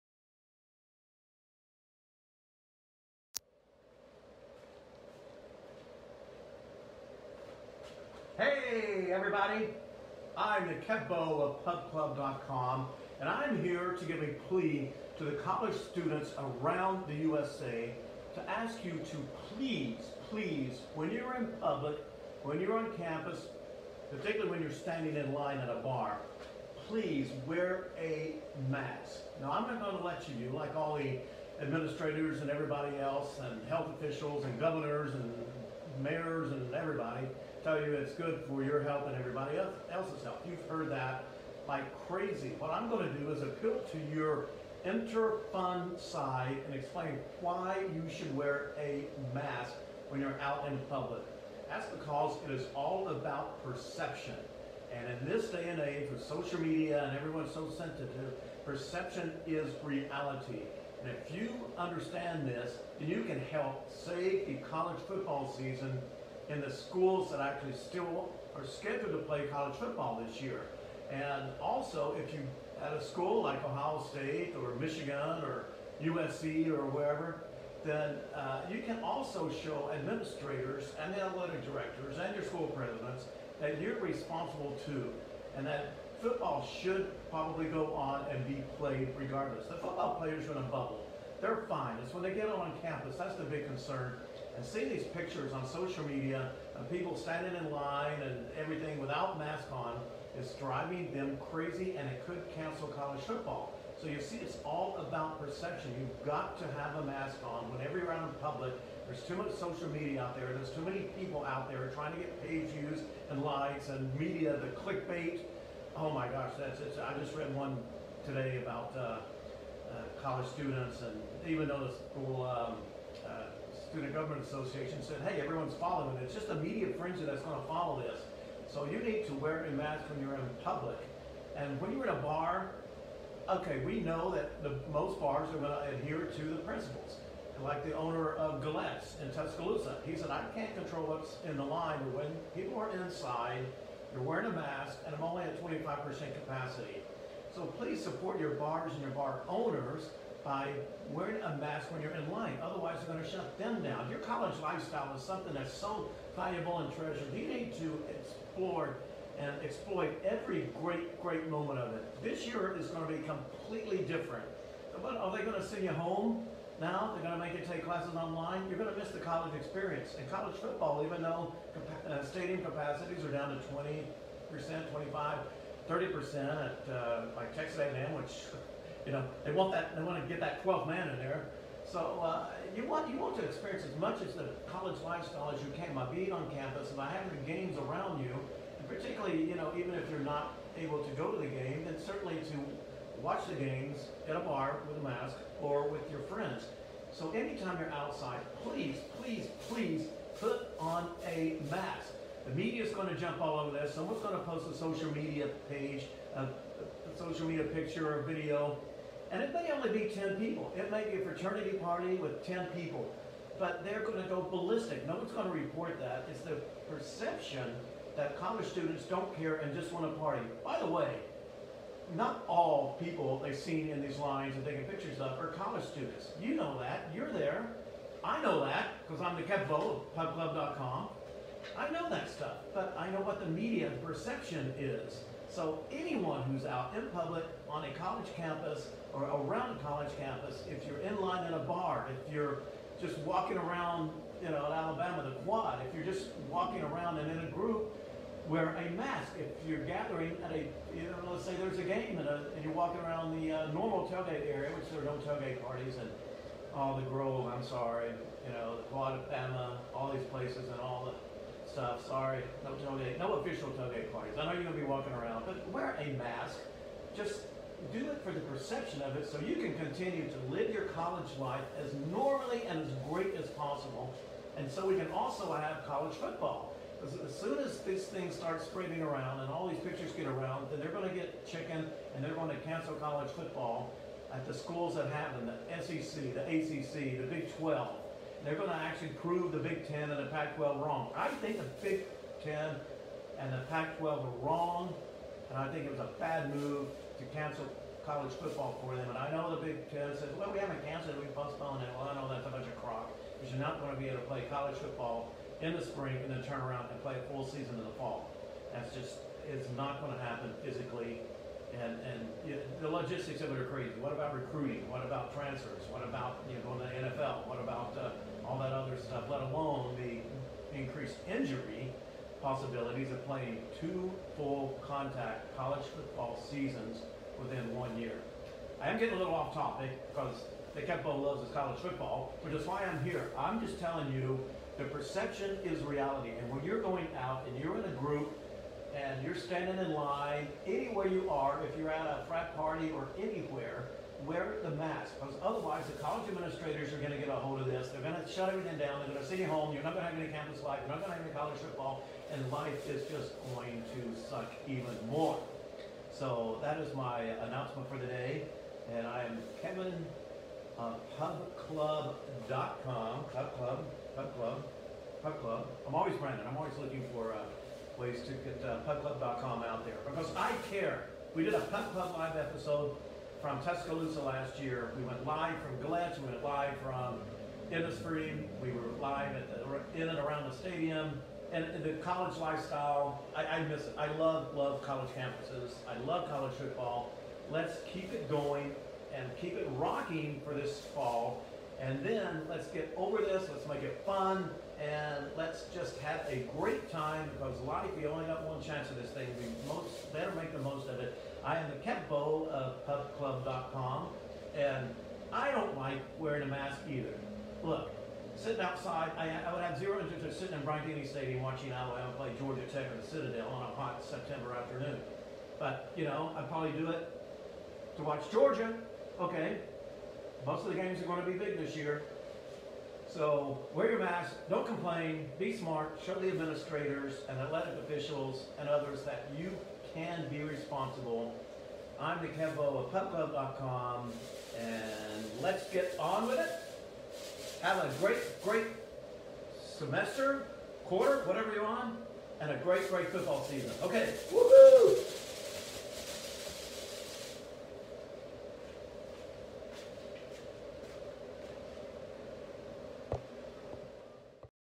A Plea For College Students To Wear A Mask (And Save College Football!)
Perception is reality, folks. You see it with mayors shutting down college bars, universities putting a halt to frat parties. All based on couple of social media photos of people not wearing masks. Do it to show you can be responsible; if not, the people in charge will freak out and cancel what's left of the college football season!